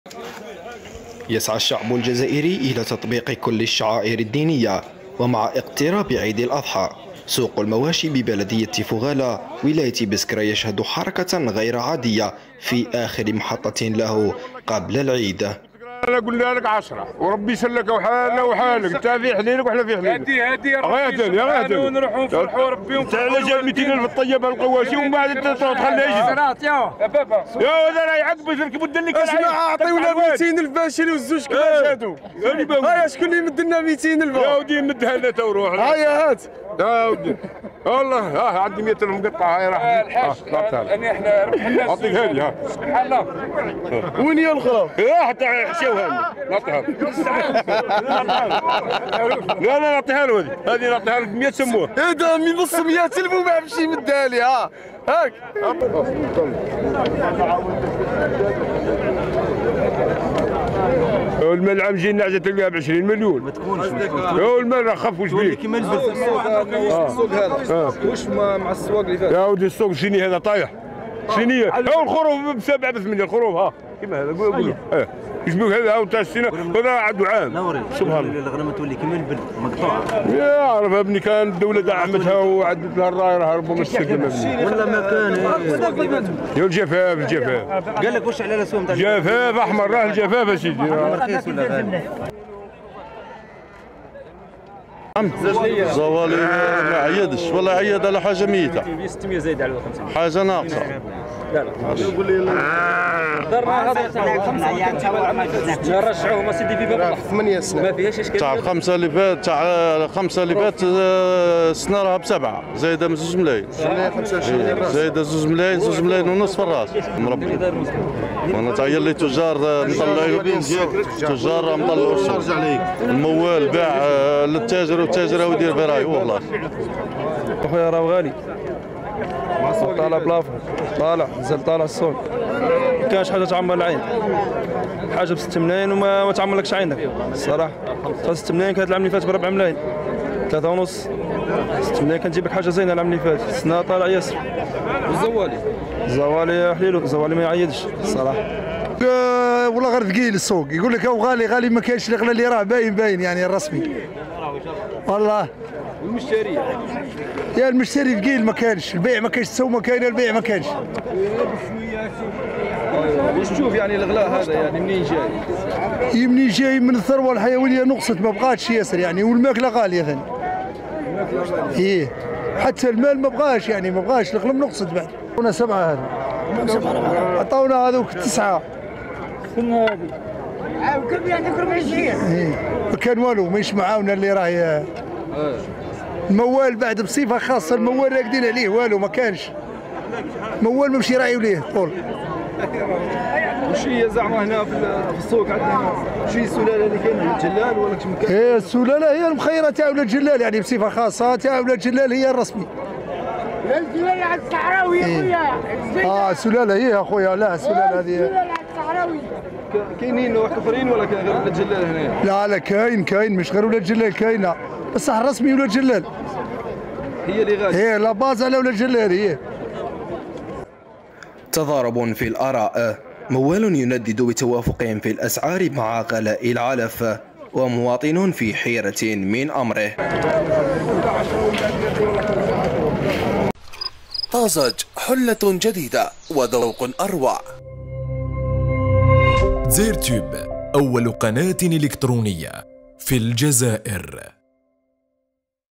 شاااهد..أسعار خيالية بسوق المواشي بولاية بسكرة ، الموالون يشتكون غلاء الأعلاف و المواطنين حاصلين
شاااهد..أسعار خيالية بسوق المواشي بولاية بسكرة ، الموالون يشتكون غلاء الأعلاف و المواطنين حاصلين و في حيرة من أمرهم